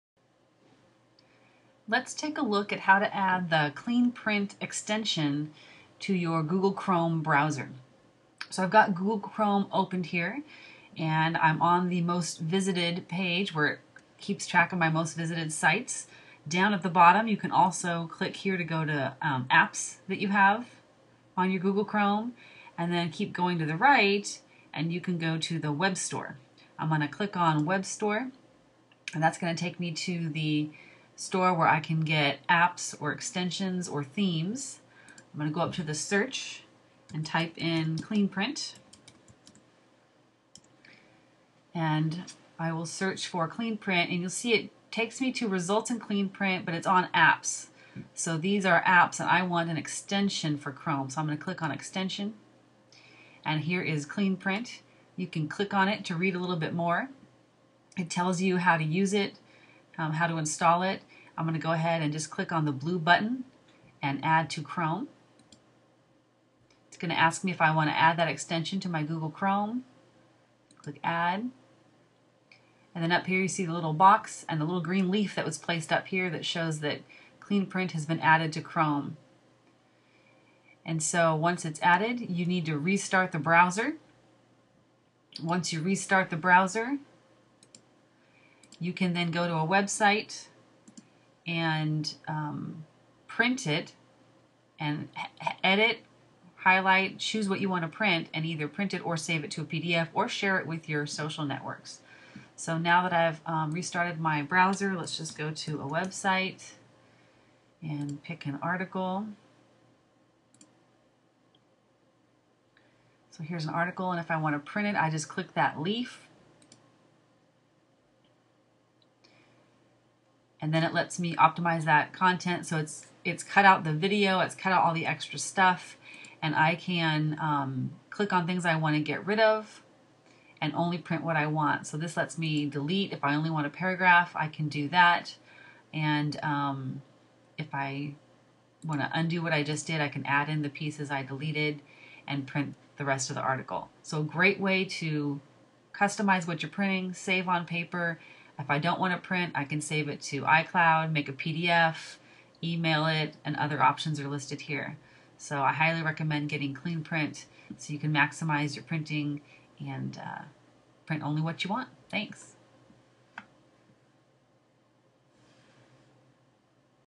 Add Clean Print Extension to Google Chrome
This Chrome extension lets you customize what your print from websites.  A great tool that also lets you save to PDF or Google Drive.